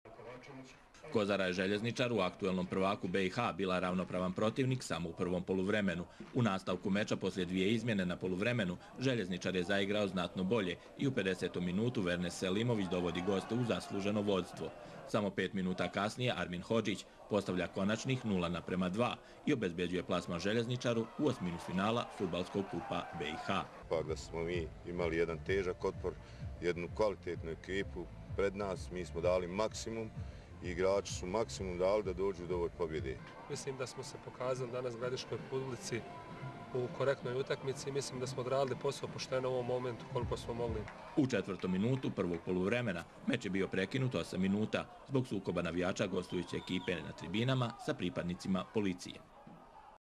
Kozara - Zeljeznicar 0:2
18. septembar 2013., 1/16 finala Kupa BiH.
Izvjestaj RTRS.